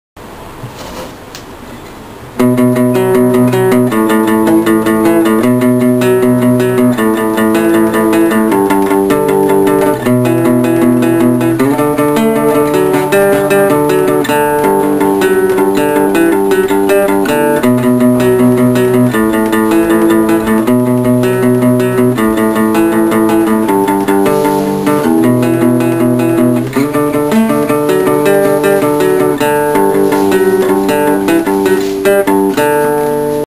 Unrealesed/Unknown song (by blink-182) covered by me
This song can be heard on the video ' I'm gay ' . The song is played in the beginning. Check it out and please comment and rate. Btw, i shot this video at night.